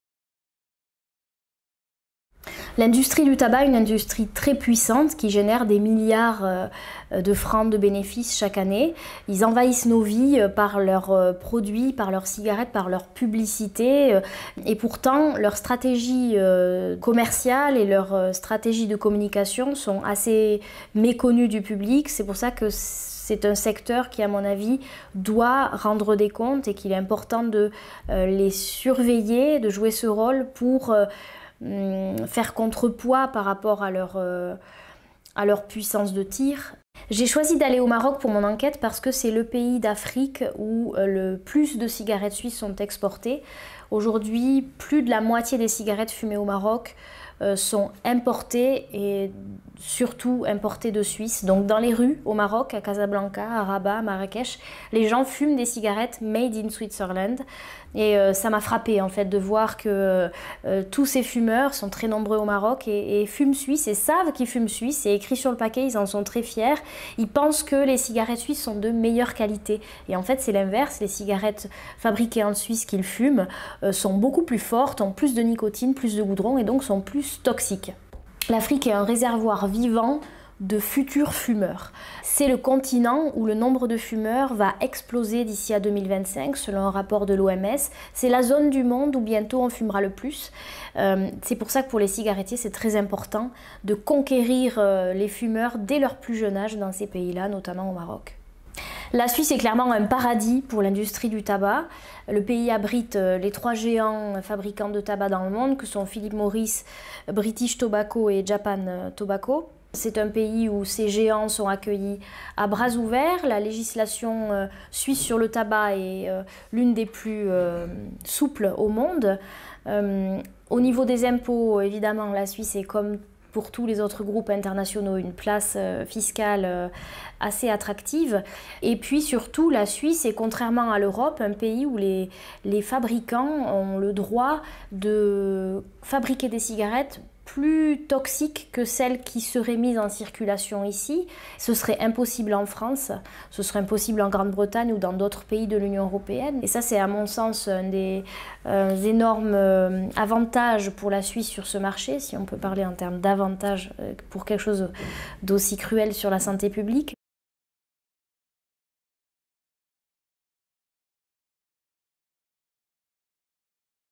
Schweizer Zigarettenfirmen in Afrika
Recherche von Marie Maurisse, Preisträgerin des Public Eye Investigation Award :
Was kaum jemand weiss: Die Schweiz exportiert mehr Zigaretten als Schokolade!
Während ihrer mehrmonatigen Recherche in Marokko deckte die freie Journalistin Marie Maurisse die Doppelstandards der hier ansässigen Tabakindustrie auf: In der Schweiz produzierte und in Afrika verkaufte Zigaretten sind viel stärker, haben ein höheres Suchtpotenzial und sind giftiger als Zigaretten, die in Europa verkauft.